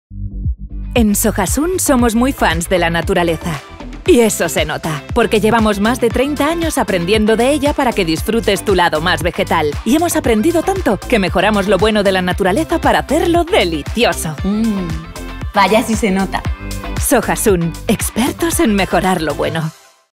Spot Sojasun - Expertos en mejorar lo bueno
¿Sabías que incrementar el conocimiento y el recuerdo de marca es uno de los objetivos más demandados por las marcas que eligen el formato video?

Es lo que pretendía Sojasun al encargarnos esta campaña, ser más visibles y reforzar su posicionamiento como expertos en productos vegetales.

Sabemos que en Sojasun son “Expertos en Mejorar lo Bueno”, concepto creativo que les posiciona como claros fans de la naturaleza.

Reforzar su posicionamiento como expertos en productos vegetales fue clave, sin embargo, no fue nuestro único objetivo perseguido.

Apoyamos a la fuerza de ventas en la apertura de nuevos puntos de distribución e incrementamos la cuota de mercado manteniendo el carácter diferencial con las marcas competidoras.

Te mostramos el Spot que creamos para alcanzar estos objetivos fijados en campaña.